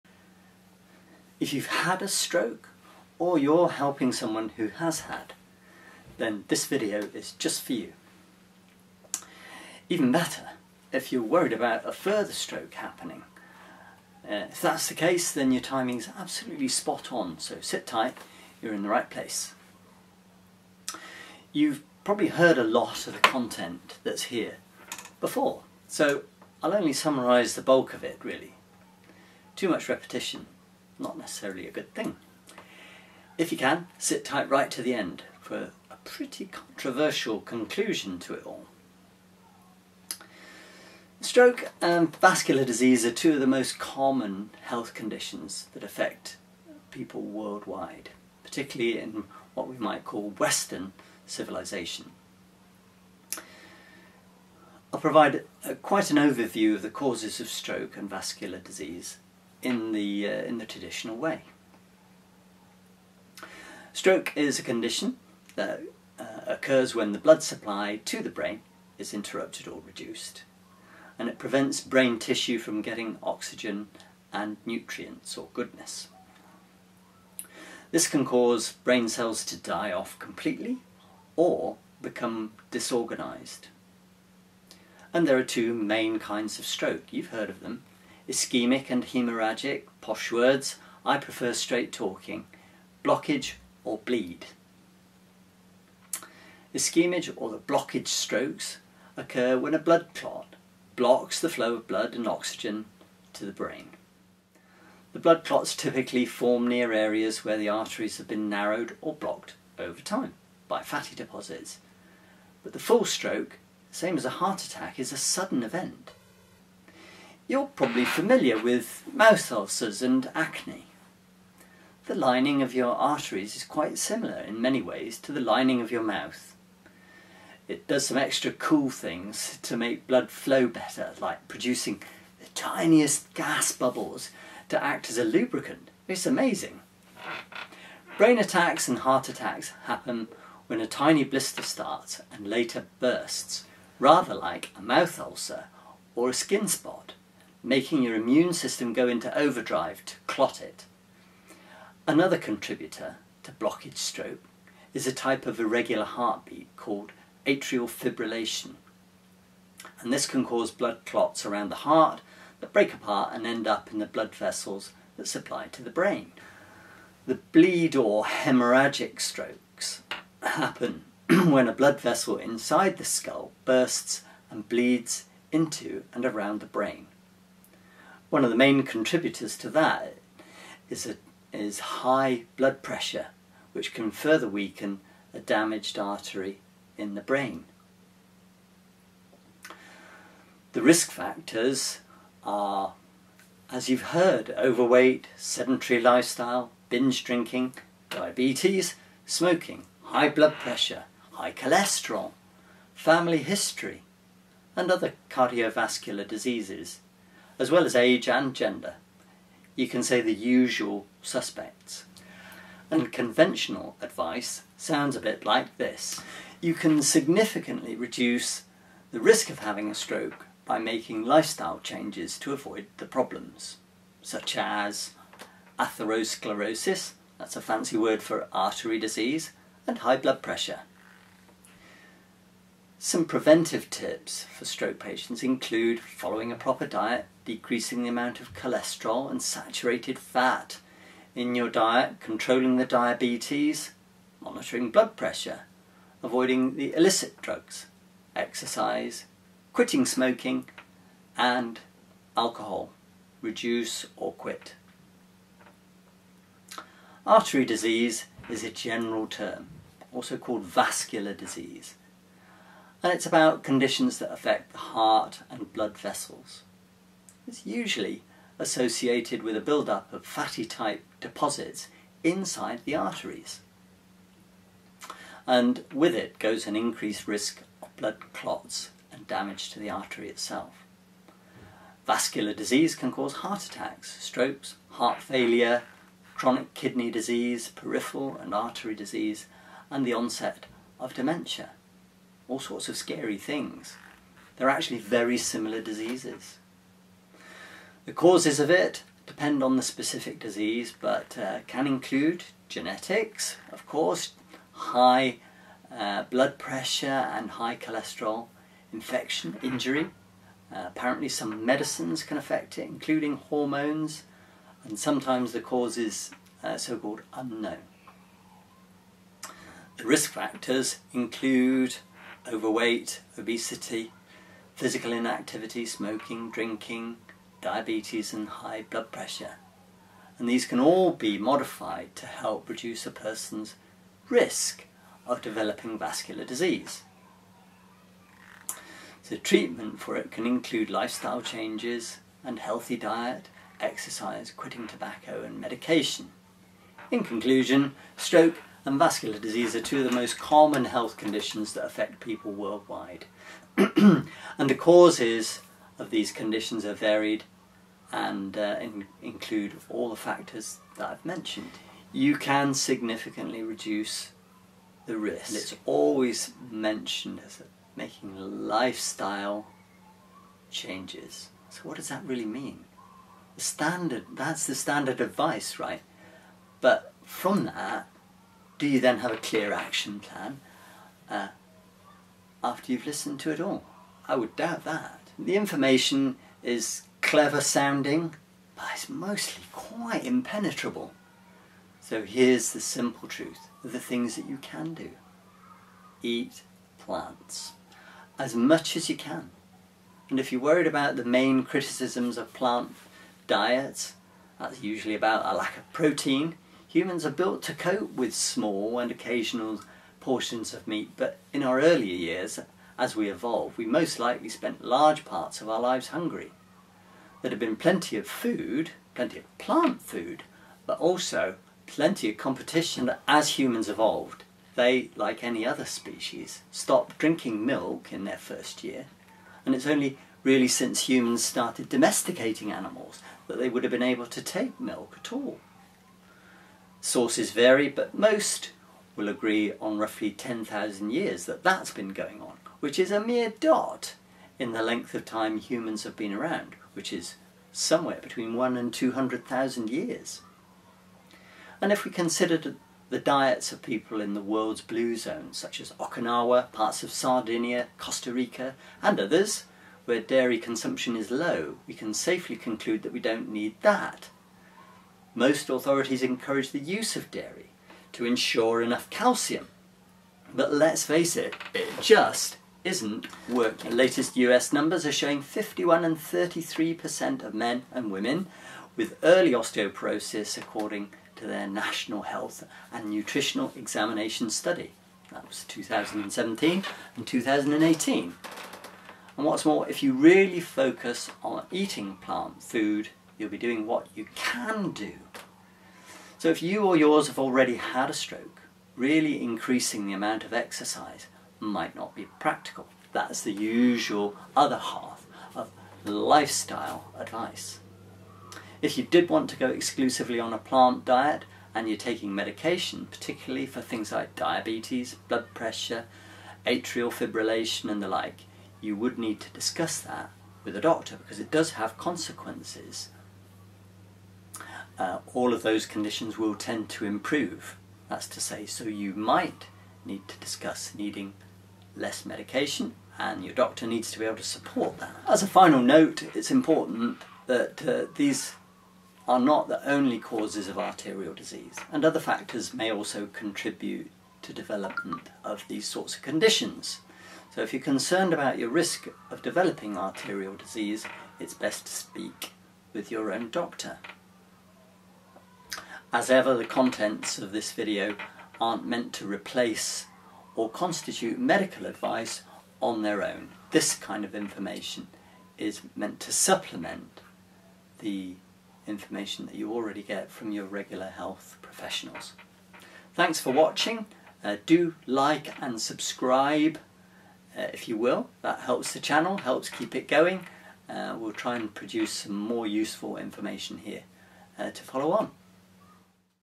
Causes of stroke
A recap on the mainstream advice, but  a different and many would say controversial conclusion.